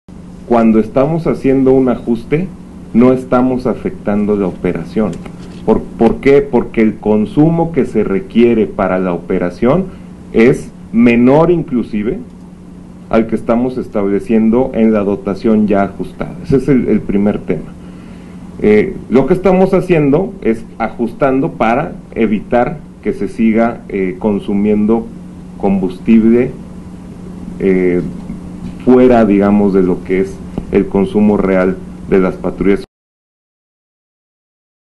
DESARTICULA ROBO DE COMBUSTIBLE
Jueves 7, marzo 2019.

DESARTICULA SSC ESQUEMA DE ROBO DE COMBUSTIBLE ASIGNADO A VEHÍCULOS

Calcula el Maestro Jesús Orta Martínez que se ahorrarán 200 millones de pesos.

Se reducirá el suministro de combustible a vehículos en 30%, pero no afectará los patrullajes; quedan exentas las ambulancias y los helicópteros del Agrupamiento Cóndores.

La Secretaría de Seguridad Ciudadana (SSC) de la Ciudad de México desarticuló un esquema de robo de combustible asignado a sus vehículos, medida con la que prevé ahorrar alrededor de 200 millones de pesos al año.

El Maestro Jesús Orta Martínez, Secretario de Seguridad Ciudadana, explicó el funcionamiento del robo, en el que participaban funcionarios de la dependencia y supervisores de las estaciones de servicio donde se abastecen las patrullas.

Detalló que se asignan 50 litros de combustible a las camionetas patrulla y 35 litros a los sedanes, pero el promedio de consumo por unidad es de 20 a 25 litros en sedanes, y de 30 a 35 en pick ups.

Los litros sobrantes, entre 15 y 20, se concentran en una tarjeta que es presentada en las estaciones de servicio y se cambia por dinero en efectivo: el 50 por ciento se lo quedan las gasolineras, y el 50%  los operadores del fraude.

Las tarjetas  permiten cargar combustible en cualquiera de las 34 estaciones asignadas para dar servicio a la SSC, pero otra forma de defraudar es con un tag que sólo permite el abastecimiento en una estación.

La SSC, por orden del Maestro Orta Martínez, disminuyó en 30 por ciento el suministro de combustible a sus vehículos oficiales (camionetas, sedanes, motocicletas y cuatrimotos), de apoyo logístico (camiones, tractocamiones, grúas y Unimog) y civiles, pero la medida no afectara a las ambulancias ni la frecuencia de los patrullajes.

“Cuando estamos haciendo un ajuste, no estamos afectando la operación, porque el consumo que se requiere para la operación es menor inclusive al que estamos estableciendo  en la dotación ya ajustada”, señaló el Secretario.

“Lo que estamos haciendo es ajustando para evitar que se siga consumiendo combustible fuera de lo que es el consumo real de las patrullas”, dijo.

Adicionalmente implementará medidas de ahorro consistentes en dar de baja más de 600 vehículos operativos y civiles, modelos 2009 y anteriores, y se dejará de suministrarles combustible.

Con su baja también se eliminan los gastos que generan por mantenimiento, por el pago de seguros y de derechos de control vehicular, además de que se liberan espacios para el resguardo de las unidades.

El Secretario anunció que en julio se prevé arrendar mil 500 patrullas, lo que también abarataría los costos de mantenimiento y pago de derechos, además de que reforzará el esquema de Cuadrantes.